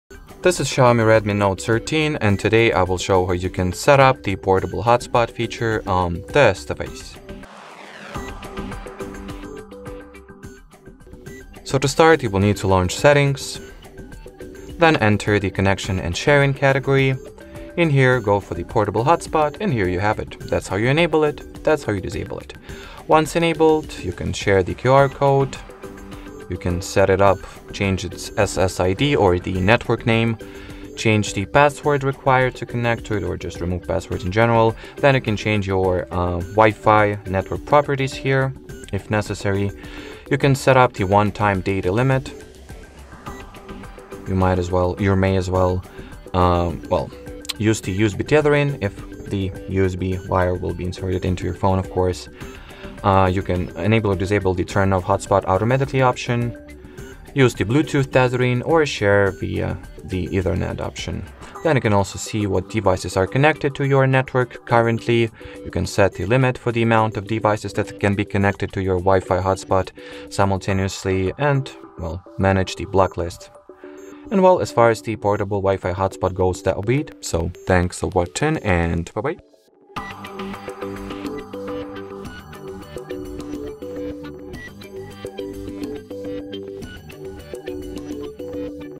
How To Set Up Portable Hotspot In XIAOMI Redmi Note 13
In this tutorial, we'll walk you through the process of setting up a portable hotspot on your XIAOMI Redmi Note 13.

What is a portable hotspot, and how does it allow users to share their device's internet connection?
Can you explain the benefits of setting up a portable hotspot on the XIAOMI Redmi Note 13?
What are the prerequisites for setting up a portable hotspot on the XIAOMI Redmi Note 13?
Can you outline the steps involved in configuring and enabling the portable hotspot feature on the device?